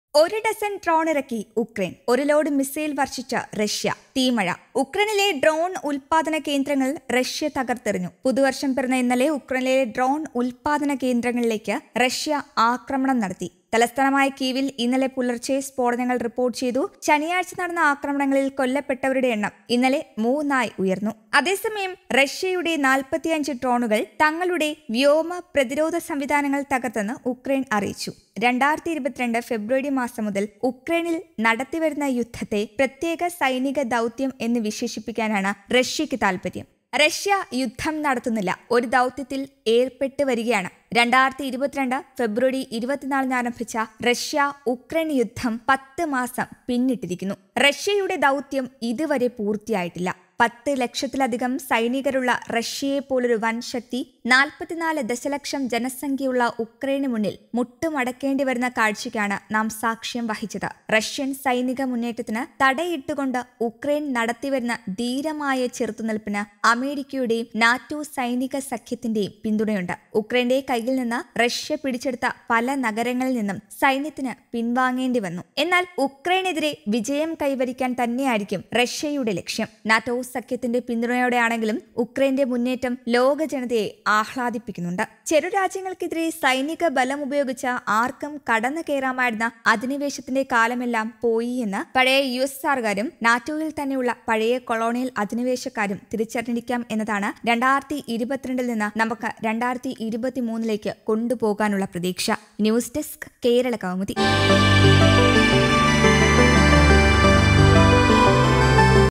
ഒരു ഡസന്‍ ഡ്രോണിറക്കി ഉക്രൈന്‍, ഒരു ലോഡ് മിസൈല്‍ വര്‍ഷിച്ച് റഷ്യ, തീമഴ | Russia Ukraine | Missiles
ഡ്രോണുകളും മിസൈലുമായി റഷ്യയും ഉക്രൈനും,​ തീമഴ

യുക്രെയിനിലെ ഡ്രോണ്‍ ഉത്പാദന കേന്ദ്രങ്ങള്‍ റഷ്യ തകര്‍ത്തെറിഞ്ഞു
പുതുവര്‍ഷം പിറന്ന ഇന്നലെ യുക്രെയിനിലെ ഡ്രോണ്‍ ഉത്പാദന കേന്ദ്രങ്ങളിലേക്ക് റഷ്യ ആക്രമണം നടത്തി. തലസ്ഥാനമായ കീവില്‍ ഇന്നലെ പുലര്‍ച്ചെ സ്‌ഫോടനങ്ങള്‍ റിപ്പോര്‍ട്ട് ചെയ്തു.ശനിയാഴ്ച നടന്ന ആക്രമണങ്ങളില്‍ കൊല്ലപ്പെട്ടവരുടെ എണ്ണം ഇന്നലെ മൂന്നായി ഉയര്‍ന്നു.